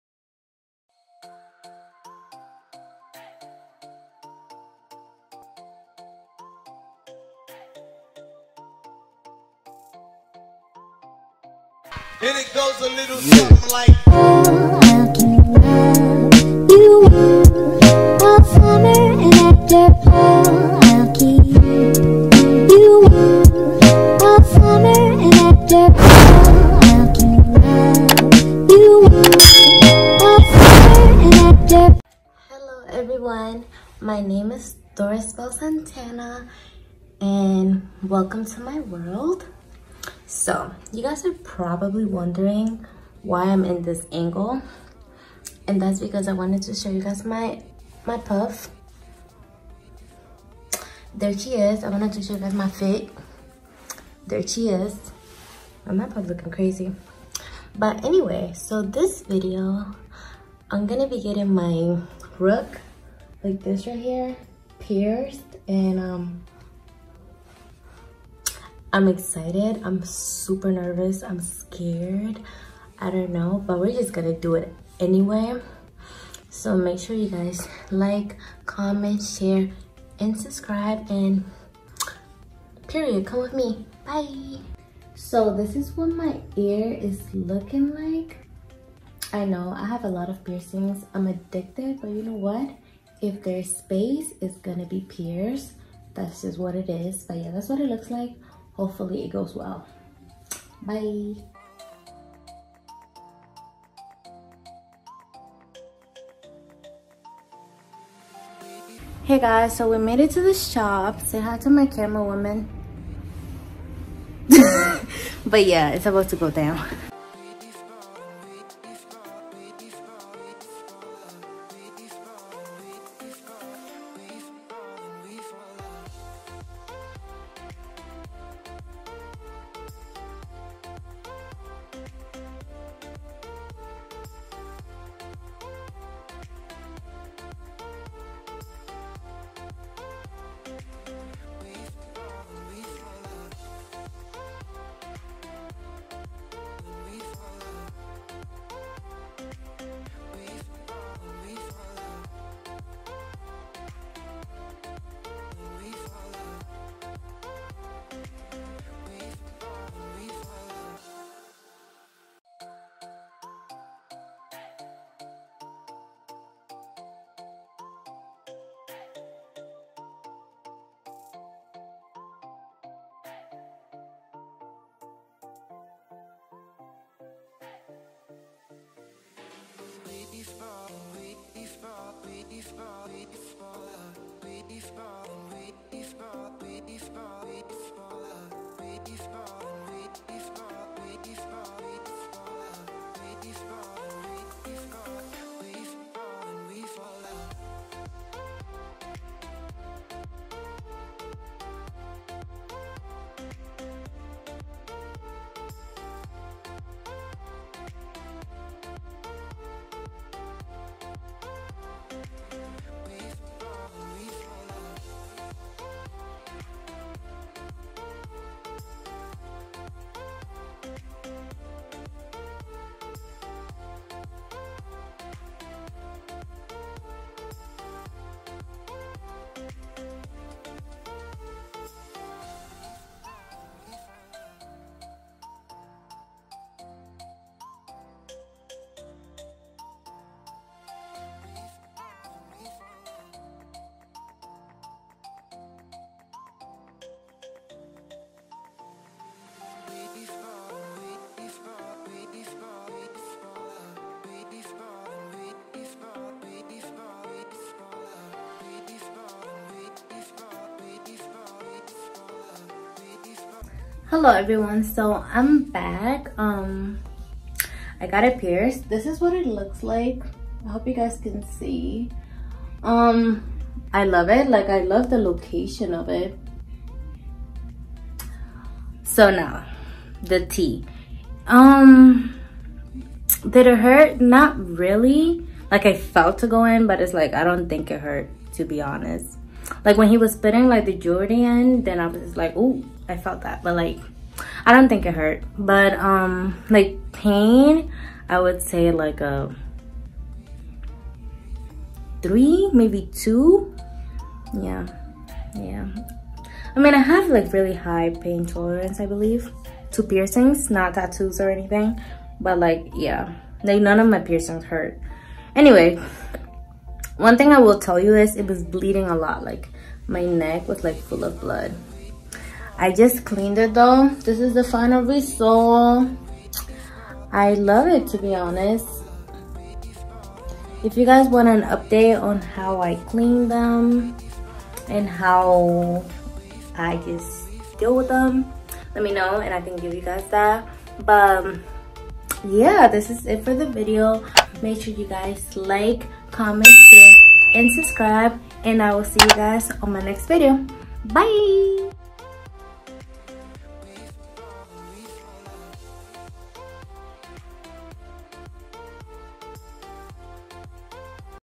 GETTING MY ROOK PIERCED!!! *Full Experience*
Hello Guyssss! Welcome to my World!

In Today’s video I will be getting my ROOK PIERCED!!! I was extremely scared, but super excited! The pain level was not too bad, it was a solid 3/10. It did bleed A LOT. Lol for a second I was scared. The first 5 days my entire ear was completely swollen, but it is all good now, after soaking it in salt water. I love it so much!! I will definitely recommend it, but just keep in mind that my entire ear was swollen for a week!

This channel will mainly Focus on Curly hair, weight loss journey, Makeup, Talks, and my Life Style!

Artist: Eiiwun (Formerly Rainlord)